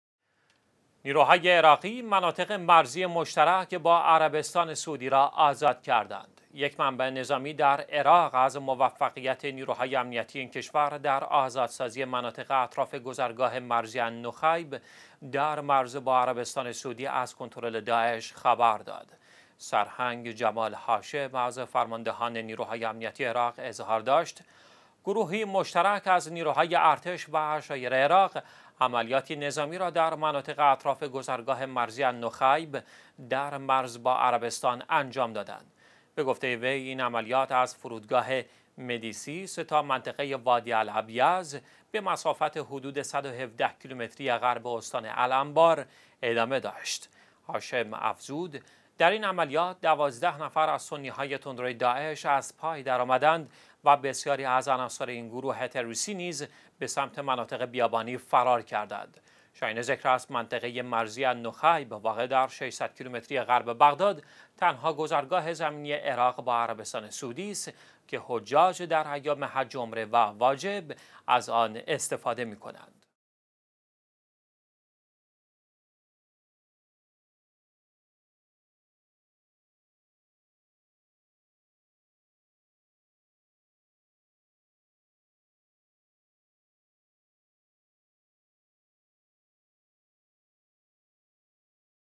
آزادسازي مناطق نزديک به مرز عراق با عربستان، از چنگ داعش
نیروهای عراقی مناطق مرزی مشترک با عربستان سعودی را آزاد کردند.

یک منبع نظامی در عراق، خبر از موفقیت نیروهای امنیتی این کشور در آزادسازی مناطق اطراف گذرگاه مرزی «النخیب» در مرز با عربستان سعودی از کنترل داعش داد.

سرهنگ «جمال هاشم» از فرماندهان نیروهای امنیتی عراق اظهار داشت: گروهی مشترک از نیروهای ارتش و عشایر عراق، عملیاتی نظامی را در مناطق اطراف گذرگاه مرزی «النخیب» در مرز با عربستان سعودی انجام دادند.

به گفته وی، این عملیات از فرودگاه «مدیسیس» تا منطقه «وادی الابیض» به مسافت حدود ۱۱۷ کیلومتری غرب استان الانبار ادامه داشت.

هاشم افزود: این نیروها در عملیات مذکور ۱۲ نفر از سنی های تندروی داعش را از پای درآورده و تعداد زیادی از عناصر این گروه تروریستی نیز، به سمت مناطق بیابانی فرار نمودند..

شایان ذکر است، منطقه مرزی «النخیب» واقع در ۶۰۰ کیلومتری غرب بغداد، تنها گذرگاه زمینی عراق با عربستان سعودی است که حجاج در ایام حج عمره و واجب از آن استفاده می‌کنند.